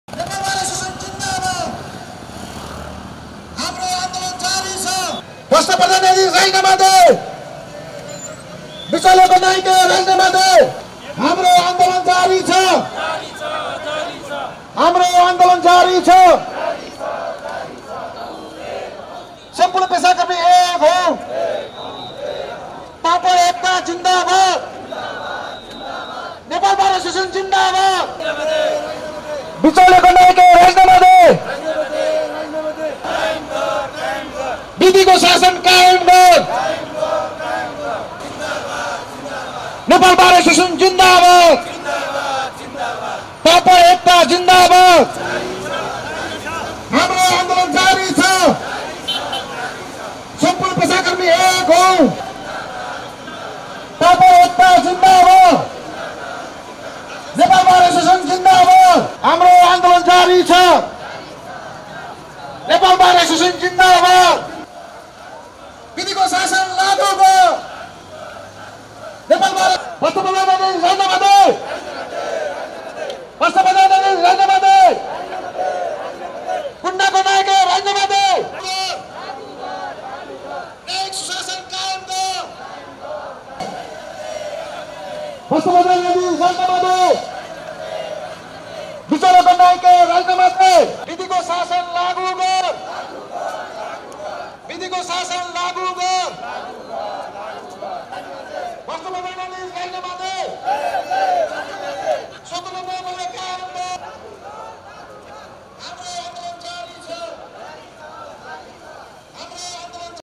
सर्वोच्चमा प्रतिगमन भयो भन्दै 'न्याय मार्च', बारको नारा
सर्वोच्चमा प्रतिगमन भयो भन्दै 'न्याय मार्च', बारको नारा : हामी अरु जान्दैनौं, चोलेन्द्रलाई मान्दैनौं